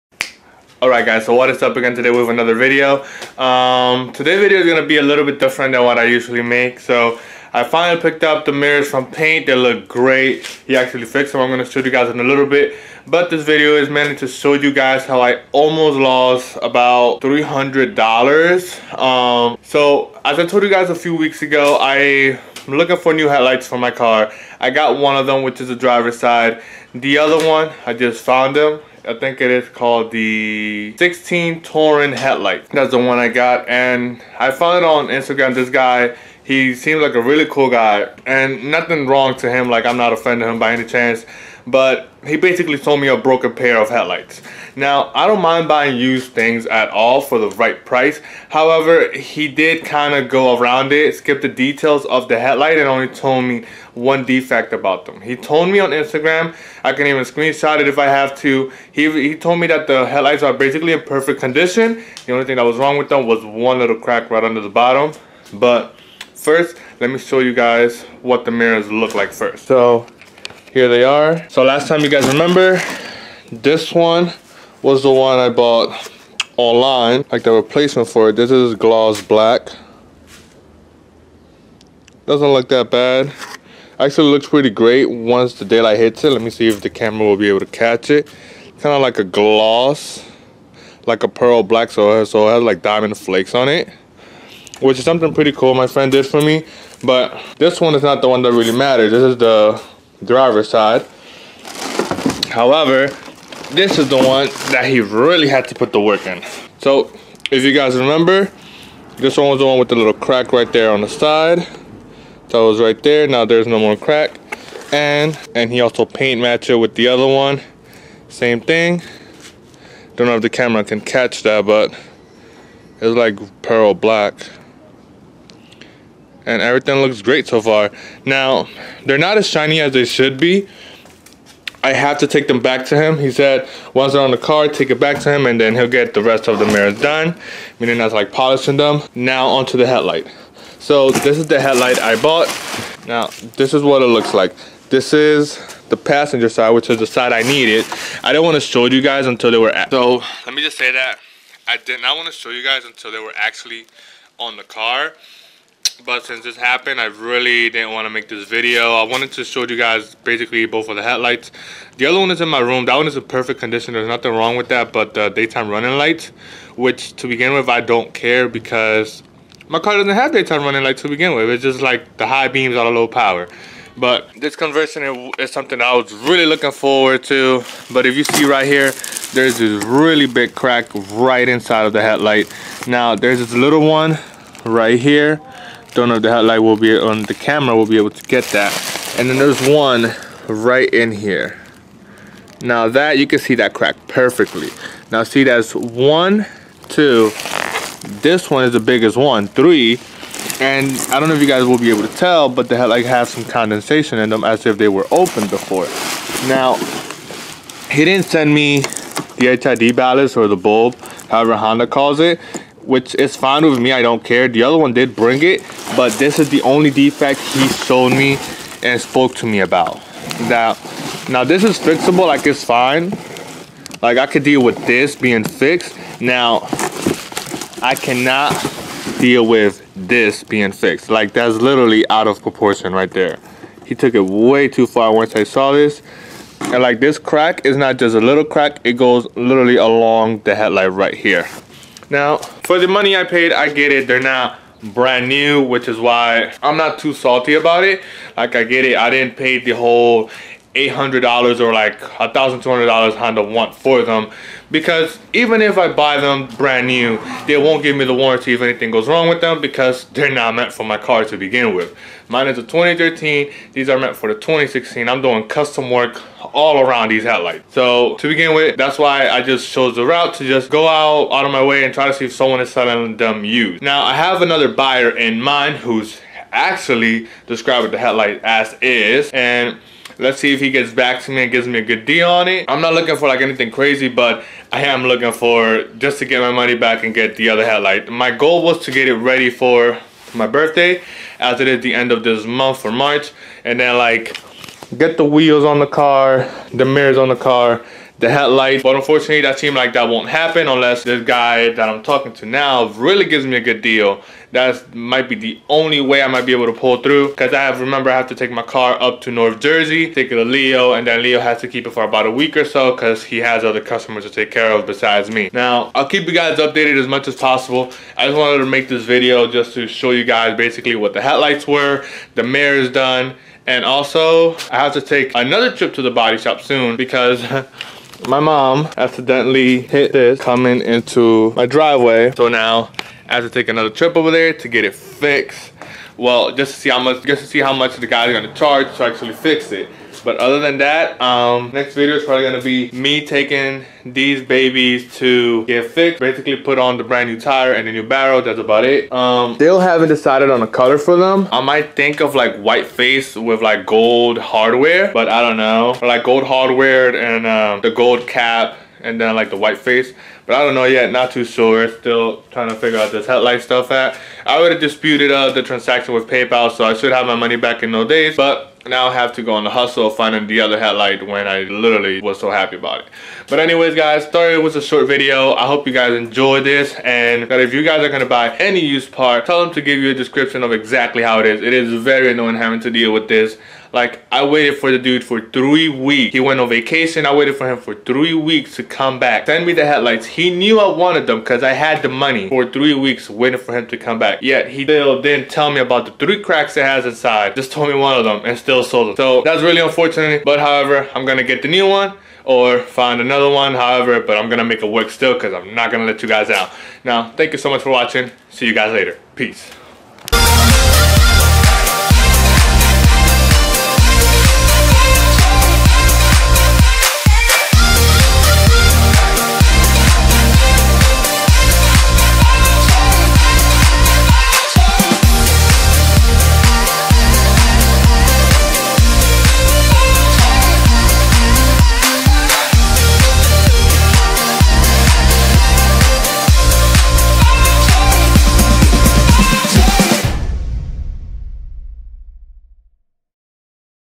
How I almost lost $300+ dollars!
Hey guys! so today I bring you guys a new video where I show you the new headlights for the Accord and also how the side mirrors came out after paint. In a few days, the damaged wheel should be getting repaired and in the meantime, I'm going to be on the search for a new headlight and also the colors that the wheels are going to be next. I hope you guys enjoy the video and I'll catch you on the next one :)